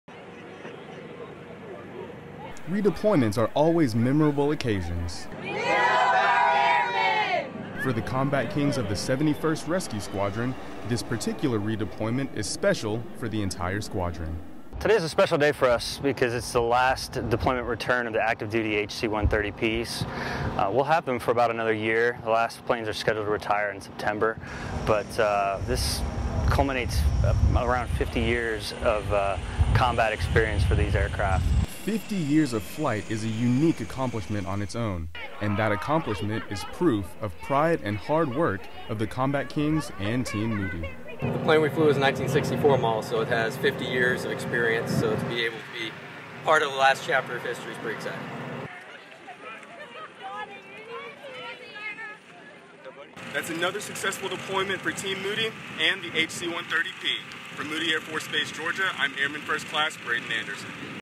71st Rescue Squadron HC-130P Last Deployment AD
The HC-130P is an aircraft with a lengthy history of flying.  A1C Braden Anderson takes us to Moody Air Force Base where a couple of HC-130Ps come home for the last time.